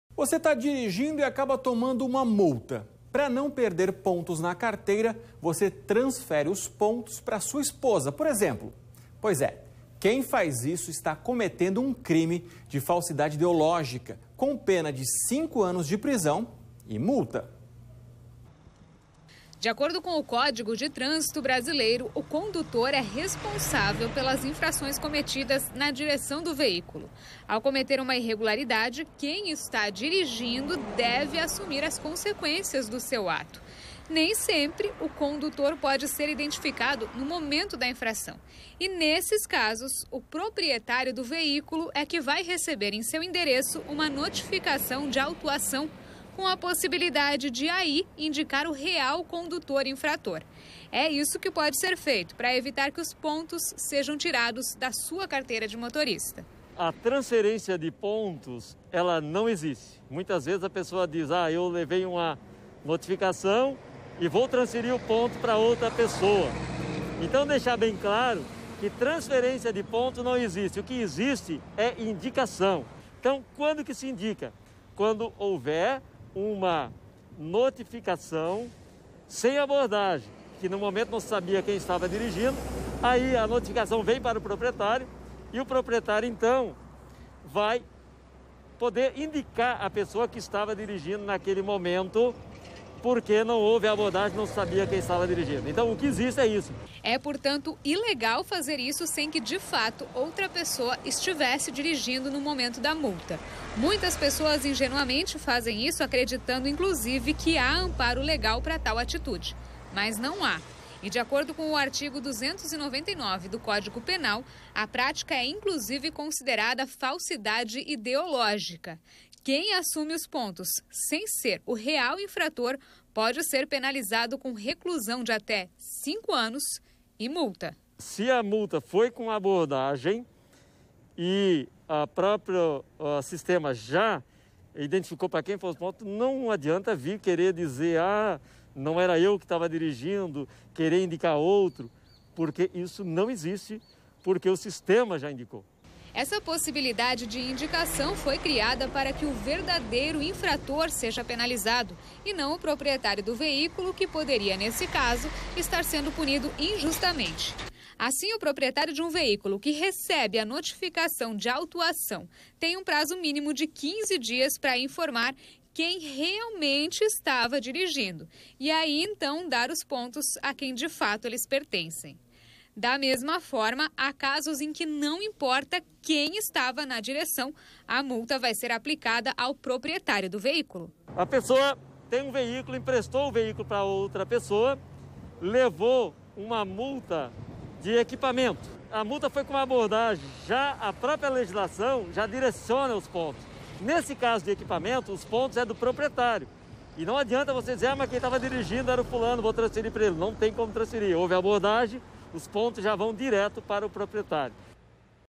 Posso Transferir Pontos da carteira de motorista?
O que diz a lei?Você está dirigindo e toma uma multa, mas para não perder os pontos na carteira, os transfere para outra pessoa. Quem faz isso está cometendo um crime de falsidade ideológica, com pena de cinco anos de prisão.